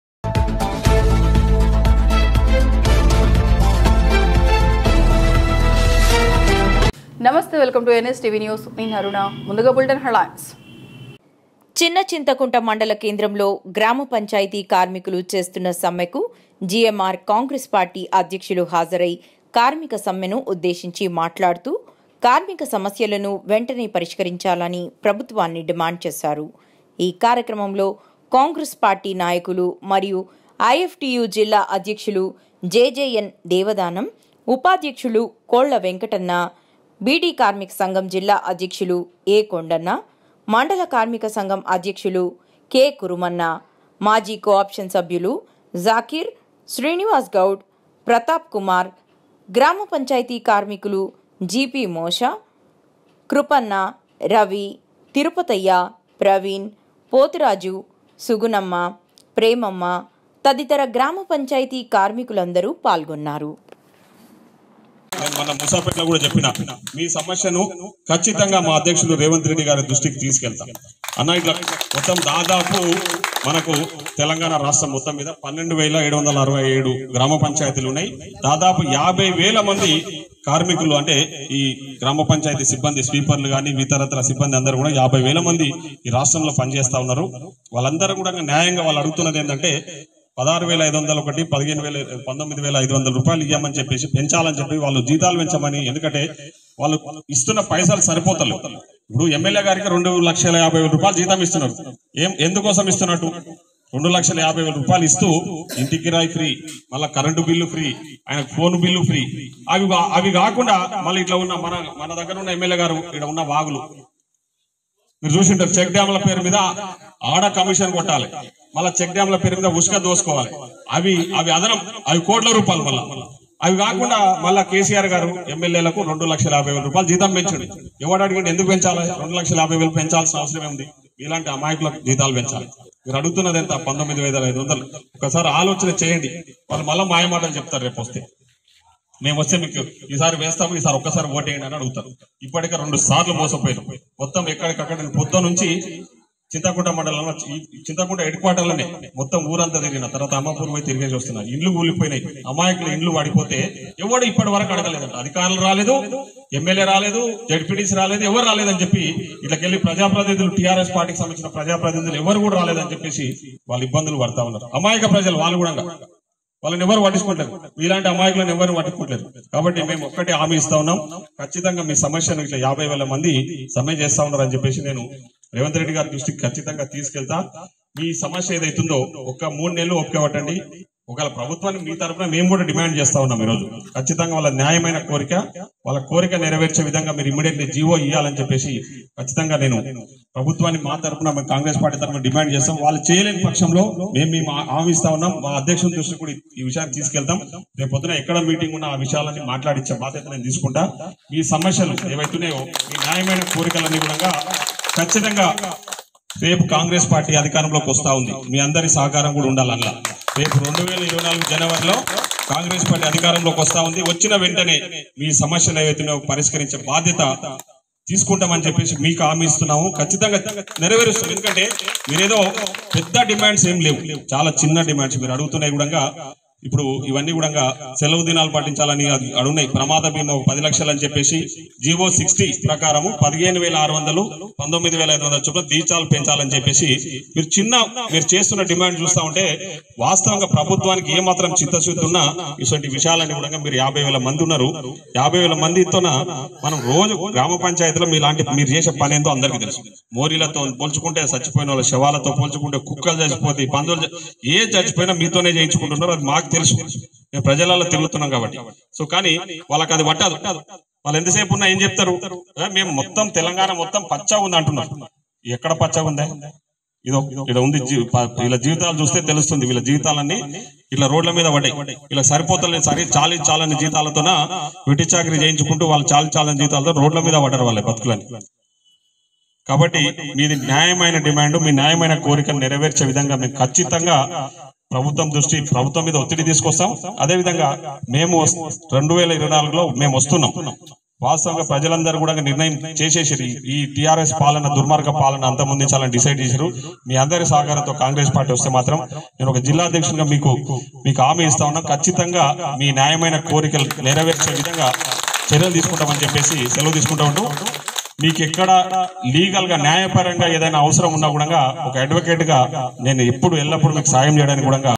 చిన్నచింతకుంట/ గ్రామపంచాయతీ కార్మికులు చేస్తున్న సమ్మెకు జిఎంఆర్ కాంగ్రెస్ పార్టీ అధ్యక్షులు హాజరై..
చిన్నచింతకుంట మండల కేంద్రంలో గ్రామపంచాయతీ కార్మికులు చేస్తున్న సమ్మెకు జిఎంఆర్ కాంగ్రెస్ పార్టీ అధ్యక్షులు హాజరై కార్మిక సమ్మెను ఉద్దేశించి మాట్లాడుతూ  కార్మిక సమస్యలను వెంటనే పరిష్కరించాలని ప్రభుత్వాన్ని డిమాండ్ చేశారు

ఈ కార్యక్రమంలో కాంగ్రెస్ పార్టీ నాయకులు మరియు ఐఎఫ్టియు జిల్లా అధ్యక్షులు జే జె ఎన్ దేవదానం ఉపాధ్యక్షులు కోళ్ల వెంకటన్న బీడీ కార్మిక సంఘం జిల్లా అధ్యక్షులు ఏ కొండన్న మండల కార్మిక సంఘం అధ్యక్షులు కే కురుమన్న మాజీ కోఆప్షన్ సభ్యులు జాకీర్  శ్రీనివాస్ గౌడ్ ప్రతాప్ కుమార్ గ్రామపంచాయతీ కార్మికులు జిపి మోష గారు కృపన్న రవి తిరుపతయ్య ప్రవీణ్ పోతురాజు సుగుణమ్మ ప్రేమమ్మ తదితర గ్రామపంచాయతీ కార్మికులందరూ పాల్గొన్నారు